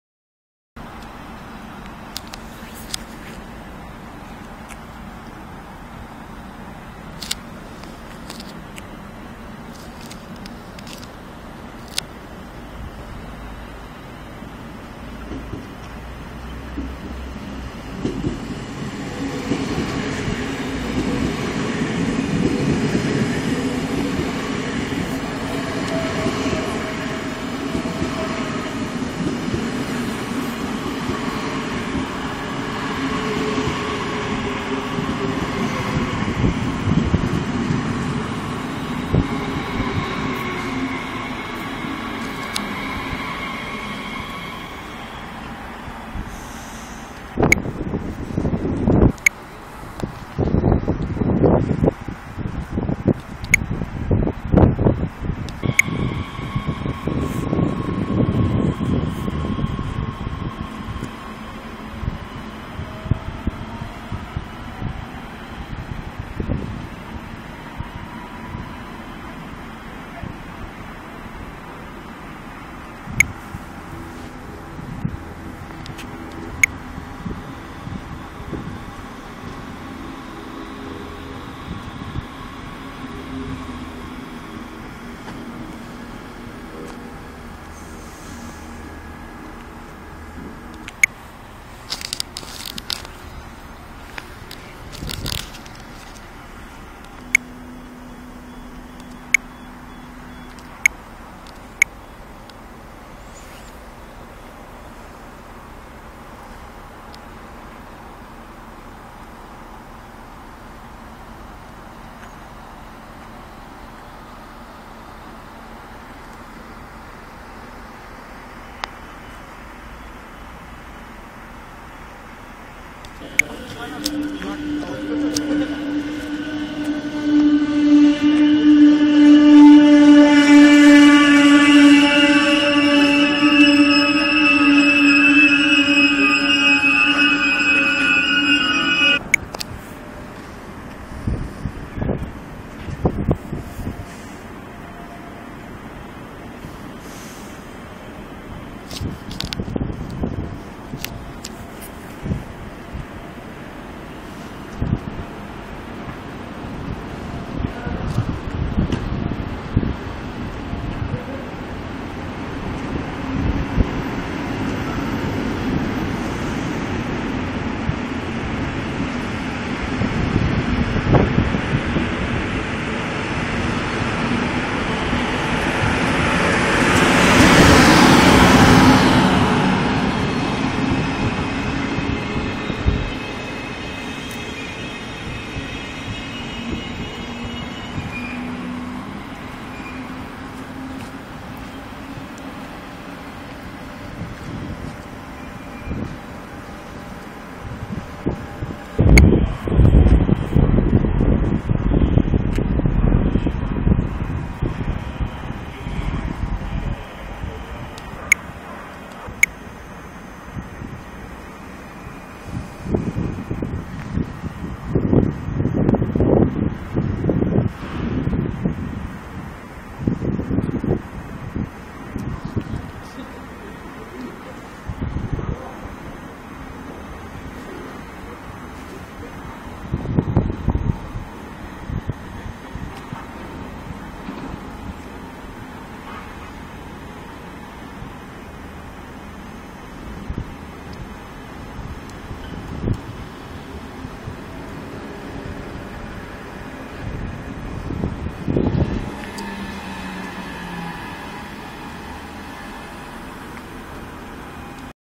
Bahnhof Plochingen mit S-Bahn Stuttgart - IC - RE
Zusehen ist die S-Bahn Stuttgart ( BR 430 / BR 423 ) RB nach Ulm Hbf (BR 143+ n-wagen) RE nach Tübingen Hbf (BR 143+Dostos)